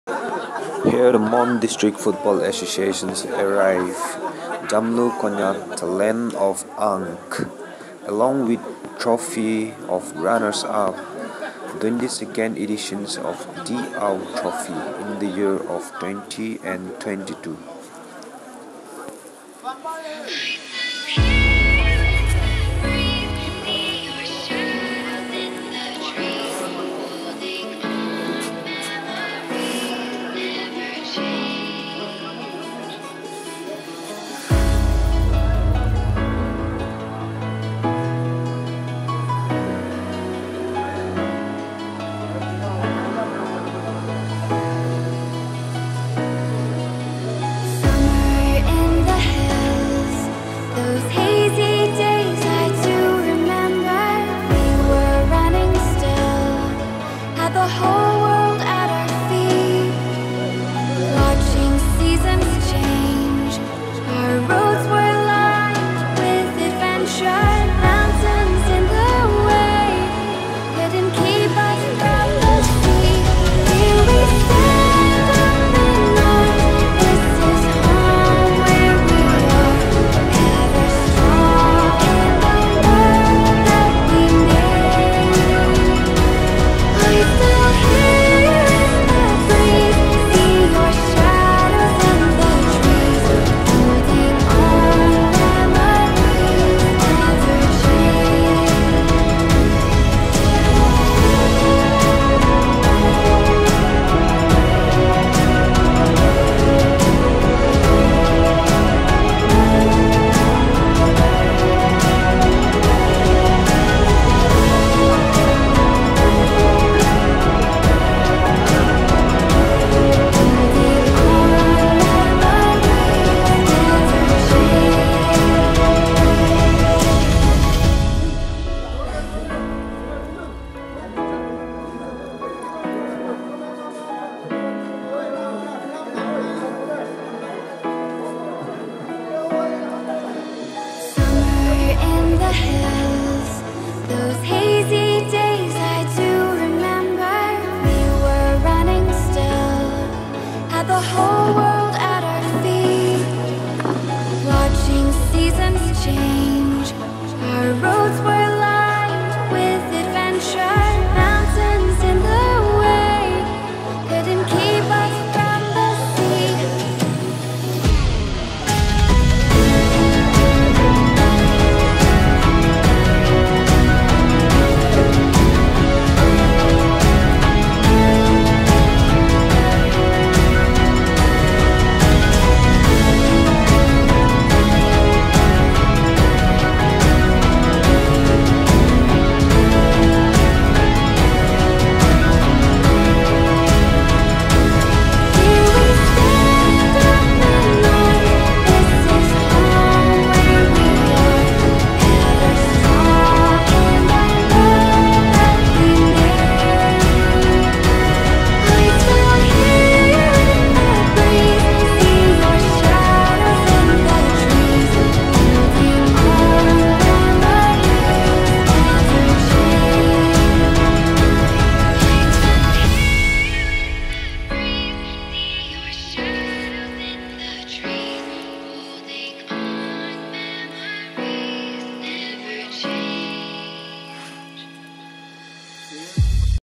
Dr.T.AO Trophy 🏆//. Mon Football Association arrived TAMLU
"LAND OF ANGH" (konyak)
The Mon Dist Football Association arrived to TAMLU THE LAND OF ANGH. along with the runners up Trophy 🏆 22nd editions of T.AO Trophy 🏆 which held in IMKONG COMPLEX STADIUM MOKOKCHUNG DISTRICT (The land of Beauty) In the year of 2022.